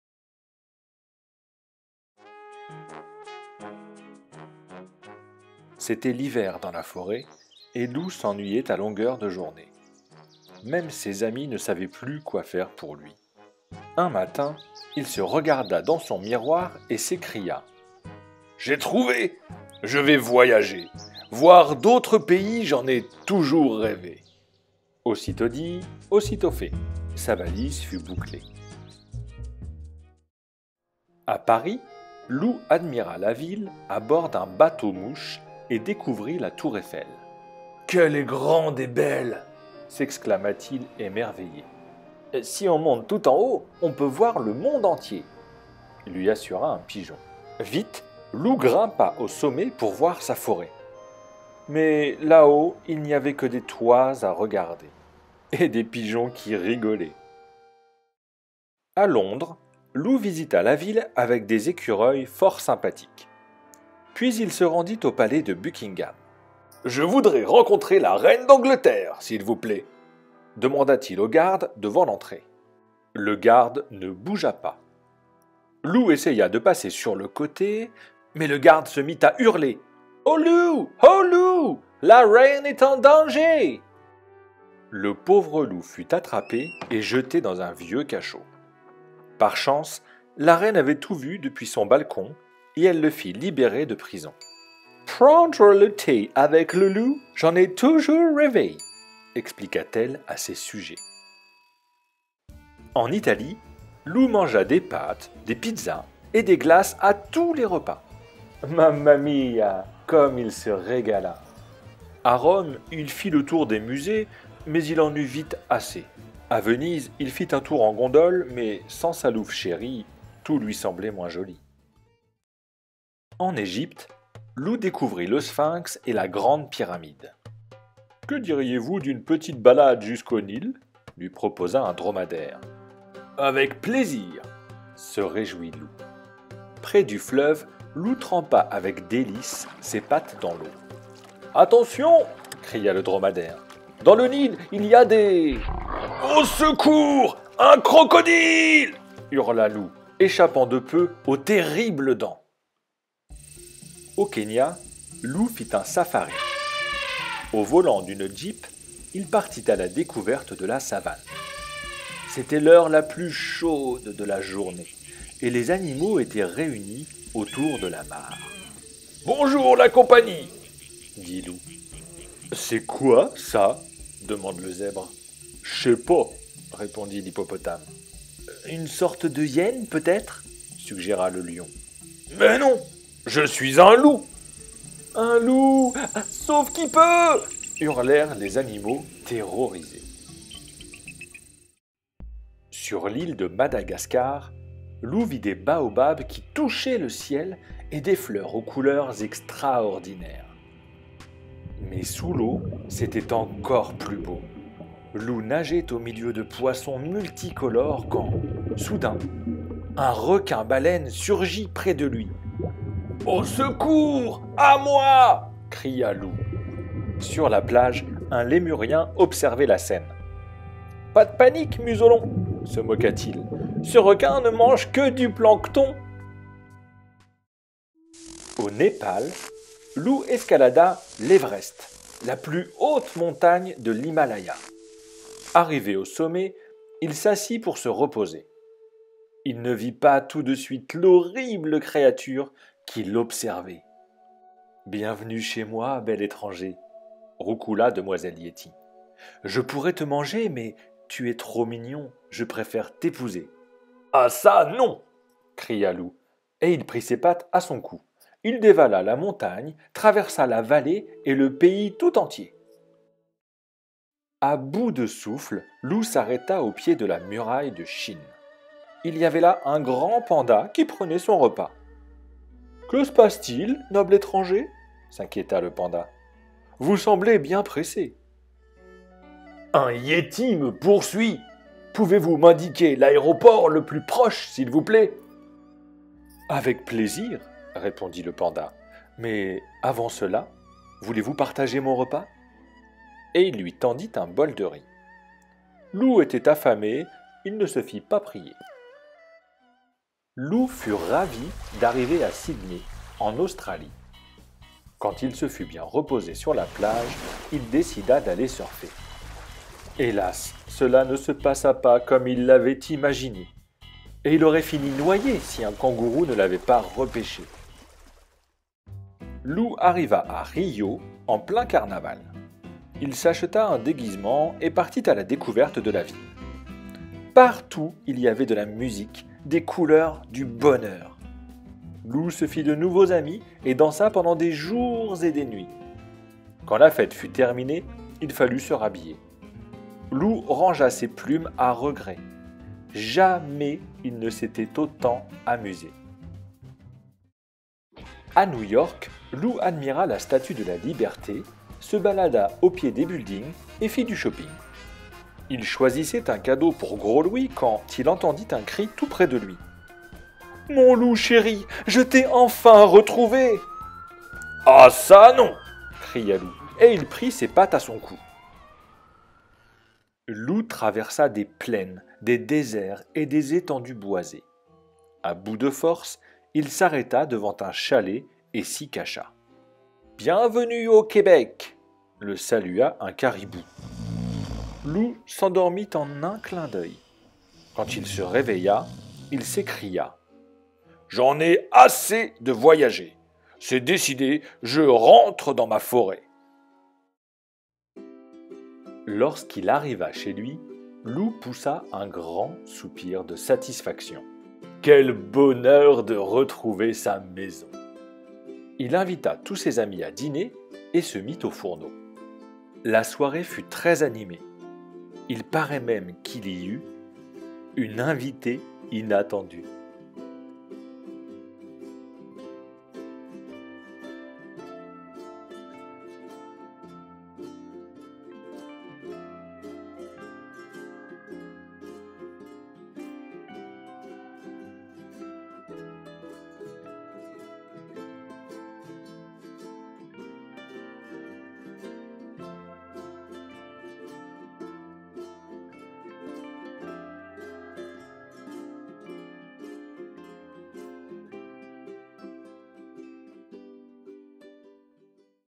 Le Loup qui voulait faire le tour du monde
Lecture de "Le Loup qui voulait faire le tour du monde" (Orianne Lallemand et Éléonore Thuillier)